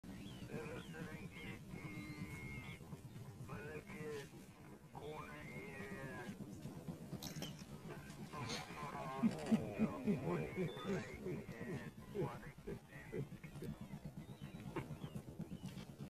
Baby Elephant Trips Up!
A baby elephant trips up trying to cross the road!